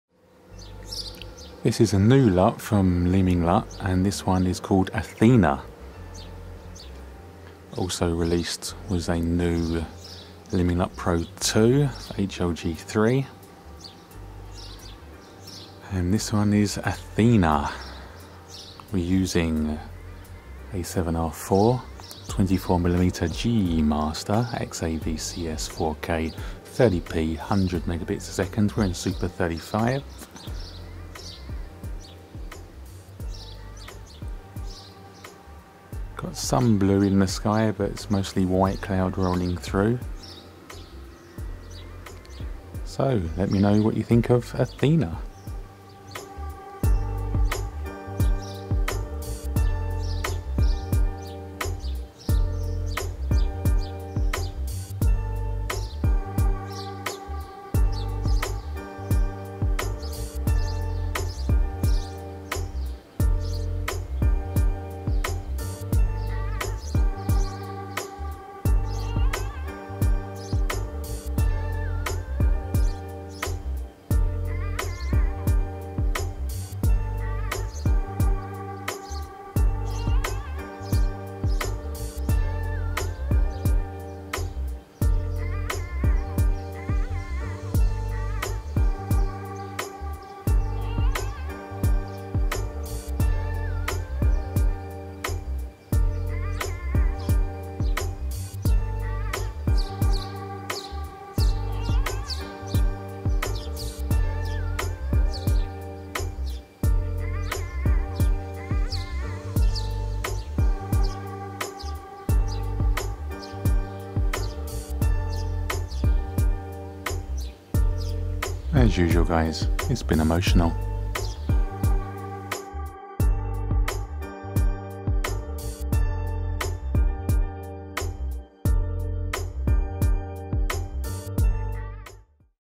Leeming LUT Athena - New For HLG3
Athena LUTs are a new, brighter version of Pro II, designed around placing middle grey (also known as 18% grey) at the 50% IRE level after the LUT has been applied, while retaining the same perfect colorimetry.

What do you prefer? Pro II or Athena?